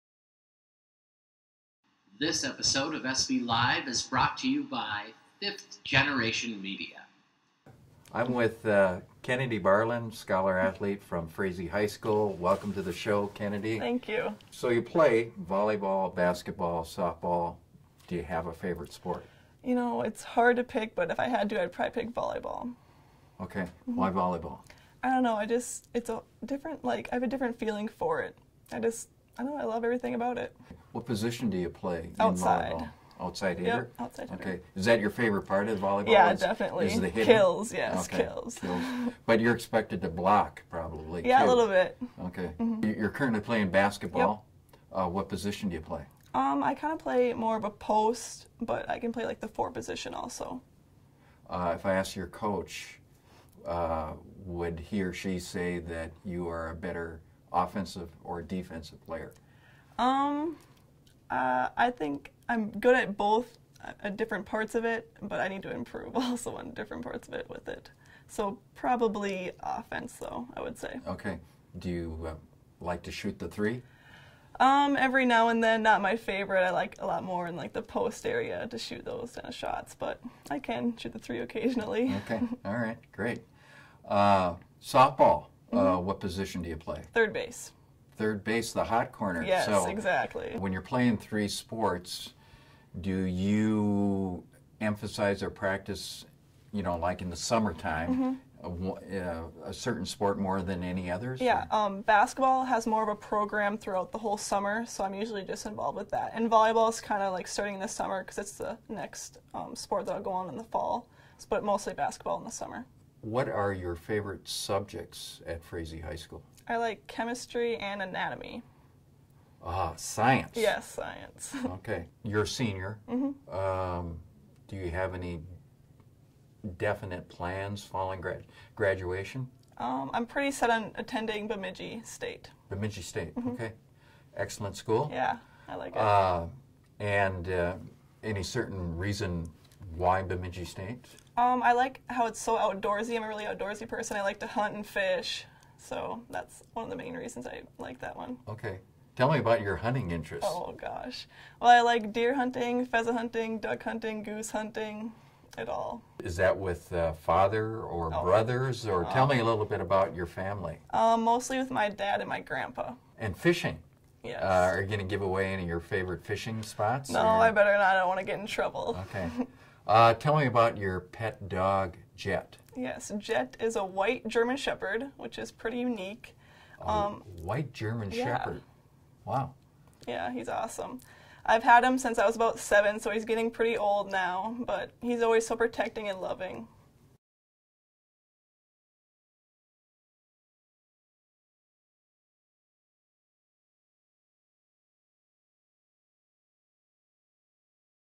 Kennady Barlund on SV Live
Frazee's Kennady Barlund lets us in on what it's like to be a 3 sport scholar athlete.